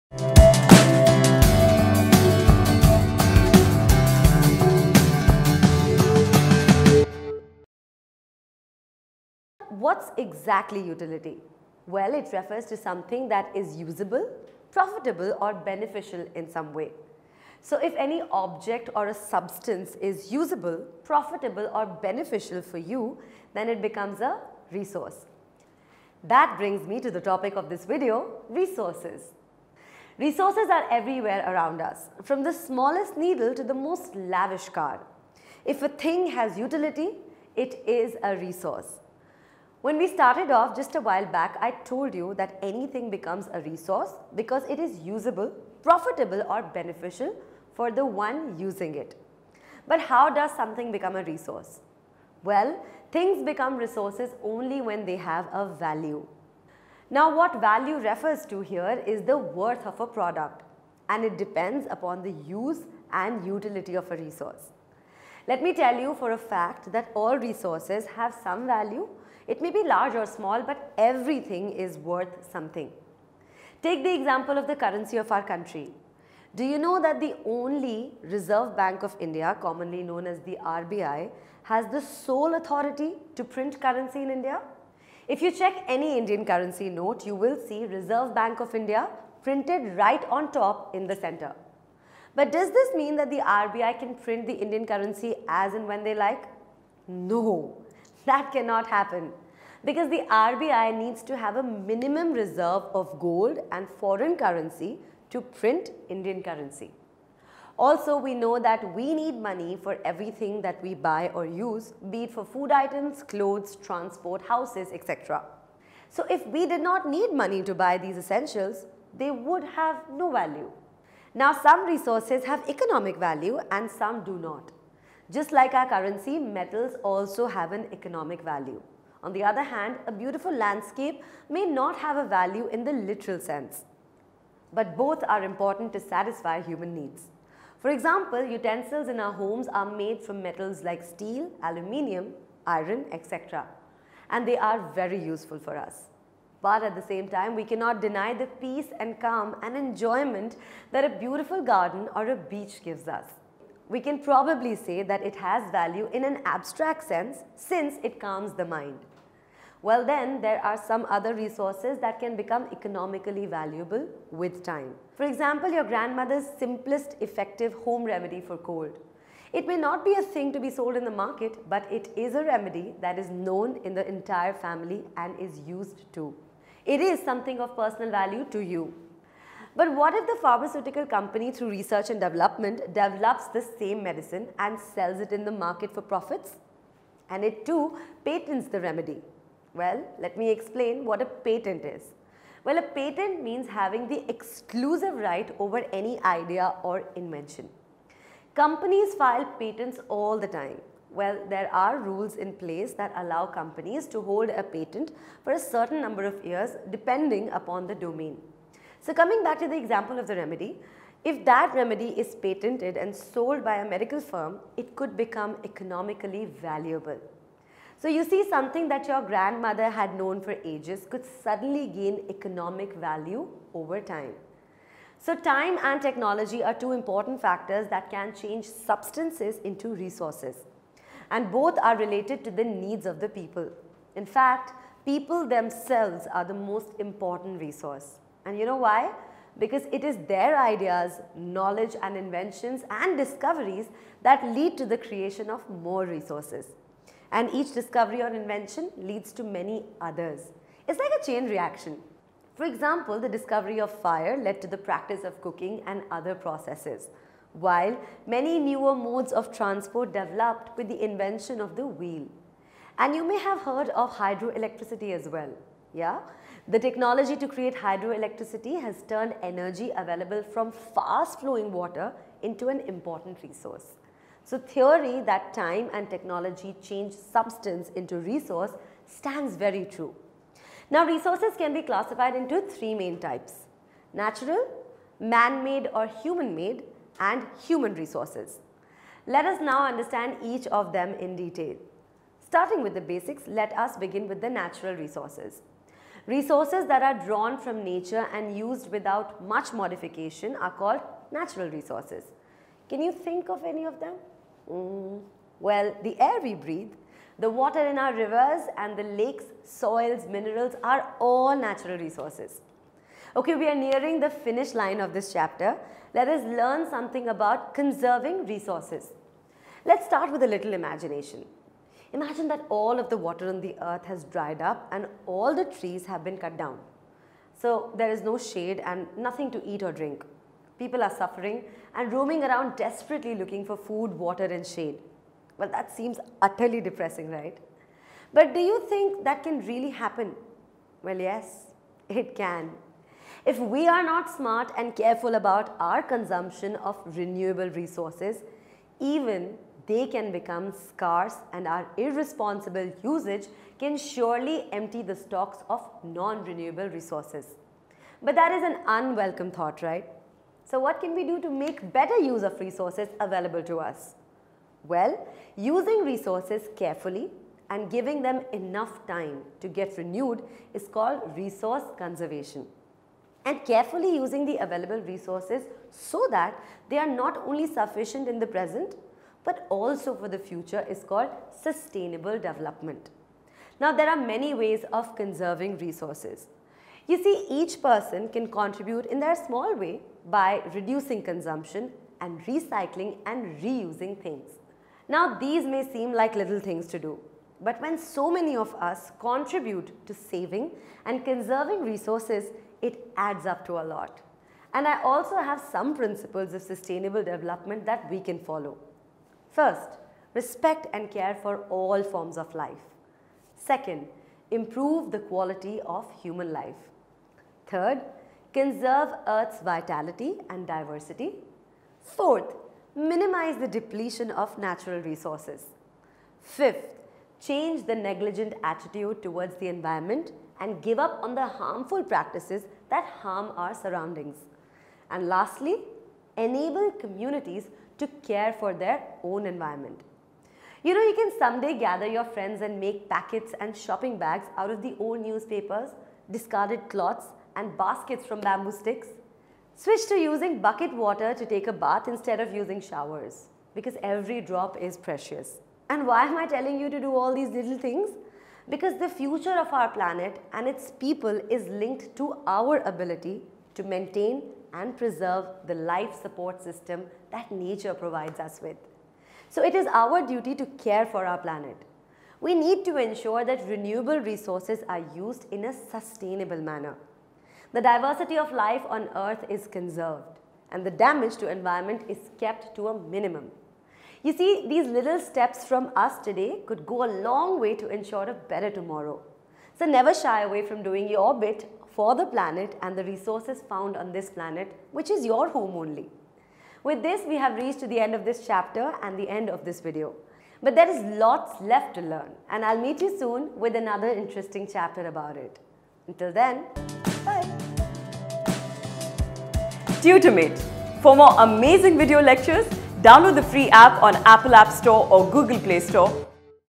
Chapter 1 Resources Geography CBSE NCERT Class 8
This is a video for the first chapter (Chapter 1) of NCERT Class 8 Geography – Resources.
This is a demo of our video lecture. You can watch the complete video by downloading the FREE TUTORMATE app on,
The video opens with an introduction and the topics covered here are:
• Natural Resources
• Human Made Resources
• Human Resources
• Conserving Resources
to learn more about the chapter ‘Resources’ and Tutor Mate.